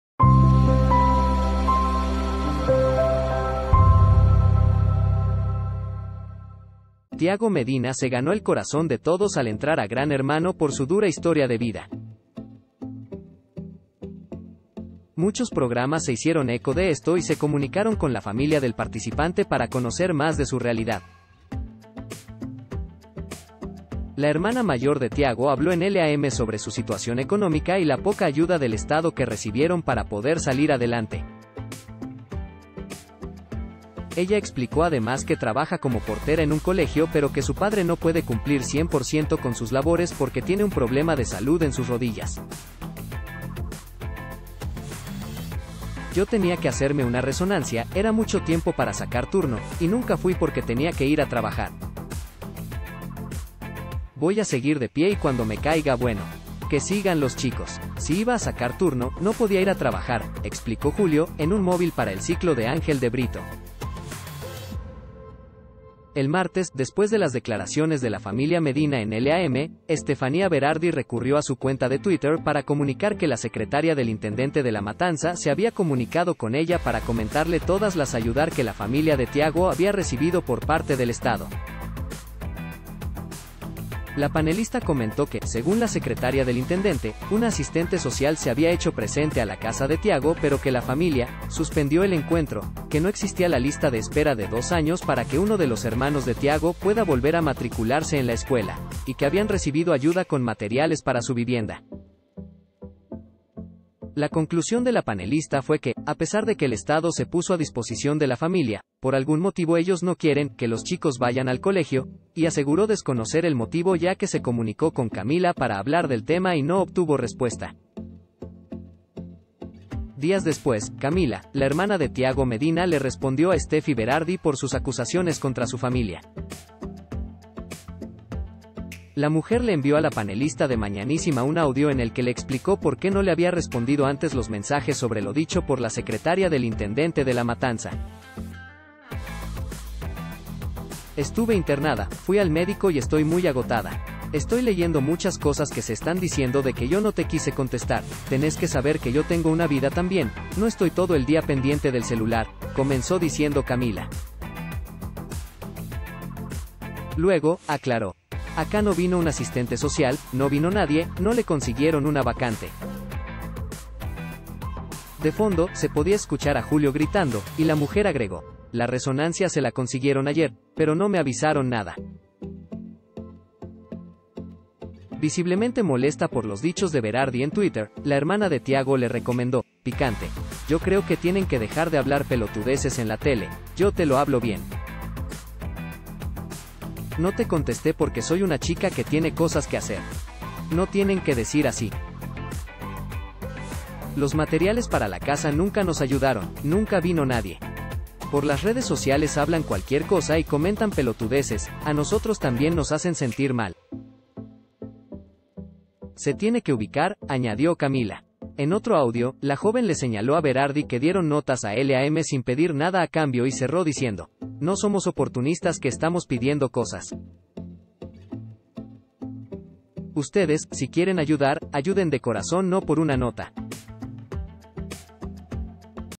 La hermana de Thiago Medina disparó contra Estefi Berardi: “Se tienen que ubicar”
Thiago Medina se ganó el corazón de todos al entrar a Gran Hermano por su dura historia de vida. Muchos programas se hicieron eco de esto y se comunicaron con la familia del participante para conocer más de su realidad.  La hermana mayor de Thiago habló en LAM sobre su situación económica y la poca ayuda del Estado que recibieron para poder salir adelante. Ella explicó además que trabaja como portera en un colegio pero que su padre no puede cumplir cien por ciento con sus labores porque tiene un problema de salud en sus rodillas.  “Yo tenía que hacerme una resonancia, era mucho tiempo para sacar turno, y nunca fui porque tenía que ir a trabajar. Voy a seguir de pie y cuando me caiga bueno… Que sigan los chicos (…) Si iba a sacar turno, no podía ir a trabajar”, explicó Julio, en un móvil para el ciclo de Ángel de Brito.  El martes, después de las declaraciones de la familia Medina en LAM, Estefanía Berardi recurrió a su cuenta de Twitter para comunicar que la secretaria del intendente de La Matanza se había comunicado con ella para comentarle todas las ayudar que la familia de Thiago había recibido por parte del Estado.  La panelista comentó que, según la secretaria del intendente, una asistente social se había hecho presente a la casa de Thiago pero que la familia “suspendió el encuentro”; que no existía la lista de espera de 2 años para que uno de los hermanos de Thiago pueda volver a matricularse en la escuela; y que habían recibido ayuda con materiales para su vivienda.  La conclusión de la panelista fue que, a pesar de que el Estado se puso a disposición de la familia, por algún motivo ellos no quieren “que los chicos vayan al colegio” y aseguró desconocer el motivo ya que se comunicó con Camila para hablar del tema y no obtuvo respuesta.  Días después, Camila, la hermana de Thiago Medina le respondió a Estefi Berardi por sus acusaciones contra su familia.  La mujer le envió a la panelista de Mañanísima un audio en el que le explicó por qué no le había respondido antes los mensajes sobre lo dicho por la secretaria del intendente de La Matanza. “Estuve internada, fui al médico y estoy muy agotada. Estoy leyendo muchas cosas que se están diciendo de que yo no te quise contestar, tenés que saber que yo tengo una vida también, no estoy todo el día pendiente del celular”, comenzó diciendo Camila. Luego, aclaró: “Acá no vino un asistente social, no vino nadie, no le consiguieron una vacante”. De fondo, se podía escuchar a Julio gritando, y la mujer agregó: “La resonancia se la consiguieron ayer, pero no me avisaron nada”.  Visiblemente molesta por los dichos de Berardi en Twitter, la hermana de Thiago le recomendó, picante: “Yo creo que tienen que dejar de hablar pelotudeces en la tele, yo te lo hablo bien. No te contesté porque soy una chica que tiene cosas que hacer. No tienen que decir así”.